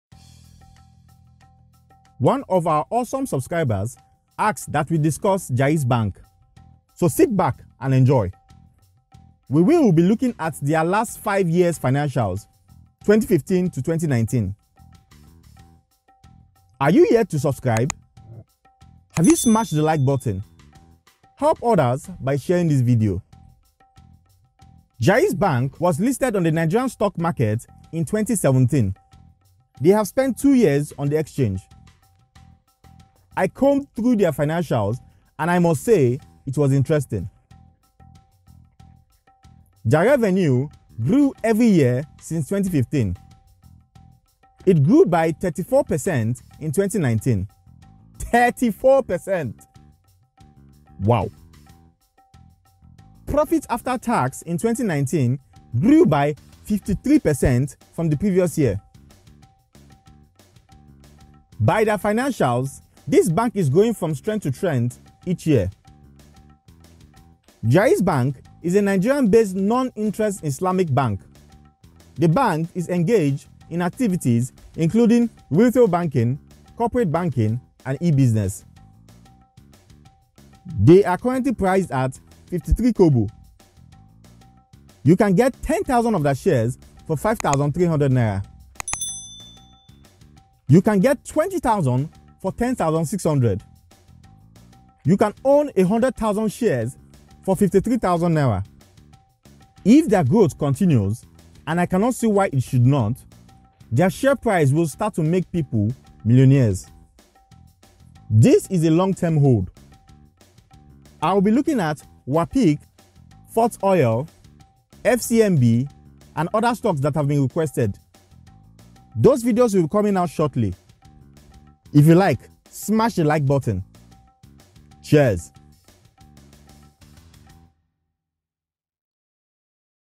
stocks to buy. Jaiz bank
This video is our opinion on Jaiz bank stocks.
We review their financials to see if they are a long term investment option.

Credits:
Photo by Andrea Piacquadio from Pexels
Photo by Charles Wundengba from Pexels
Photo by Rosemary Ketchum from Pexels